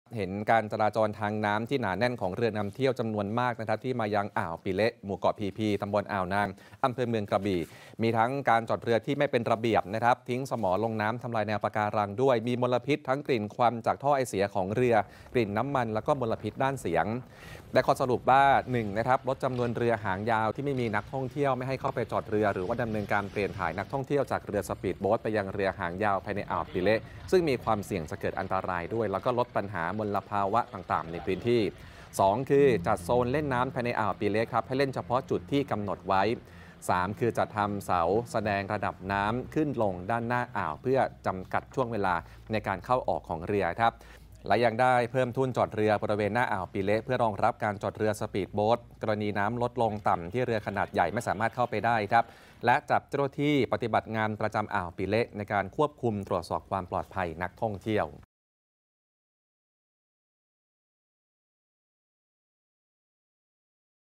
จัดระเบียบเรือนำเที่ยวอ่าวปิเละ จ.กระบี่ | Thai PBS News
แต่ละวันมีเรือนับร้อยลำเที่ยวอ่าวปิเละ จ.กระบี่ เสี่ยงต่ออันตรายและกระทบทรัพยากรธรรมชาติ จึงต้องจัดระเบียบทั้งการจราจรทางเรือและการลงเล่นน้ำของนักท่องเที่ยว